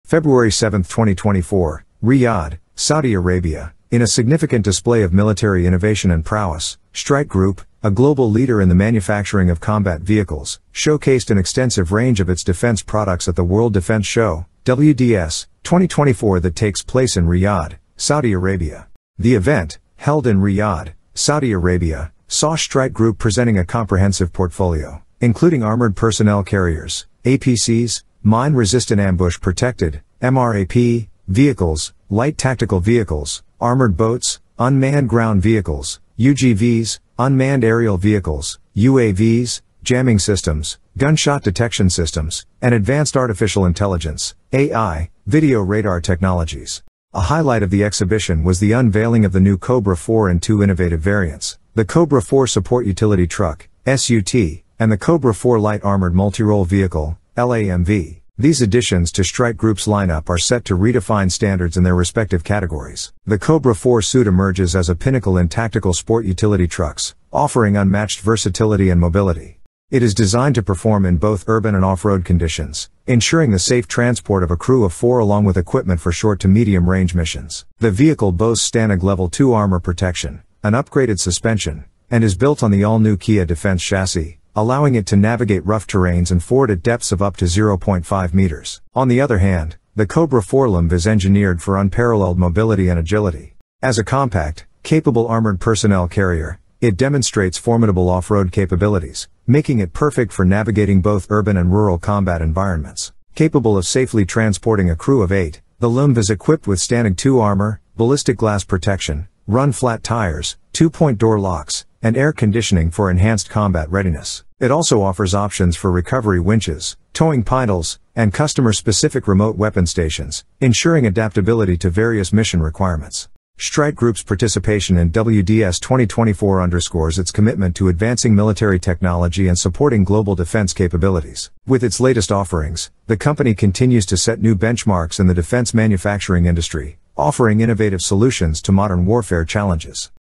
Streit Group Unveils Advanced Defense Solutions at World Defense Show 2024 in Saudi Arabia
February 7, 2024, Riyadh, Saudi Arabia – In a significant display of military innovation and prowess, Streit Group, a global leader in the manufacturing of combat vehicles, showcased an extensive range of its defense products at the World Defense Show (WDS) 2024 that takes place in Riyadh, Saudi Arabia.